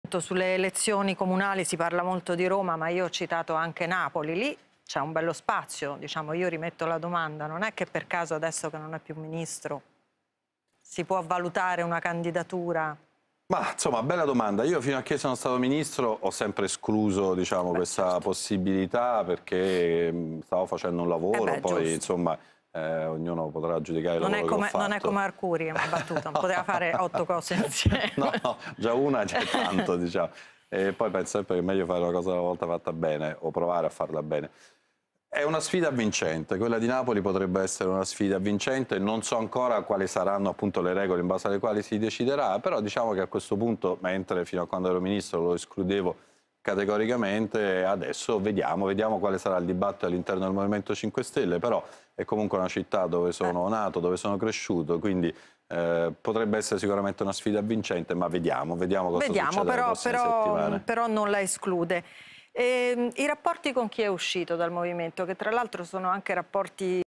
Amministrative, Vincenzo Spadafora: "Candidato a Napoli? Vediamo, potrebbe essere una sfida ...
Vincenzo Spadafora (M5S) non esclude la sua candidatura a sindaco di Napoli: "Vediamo, potrebbe essere una sfida avvincente, è la mia città"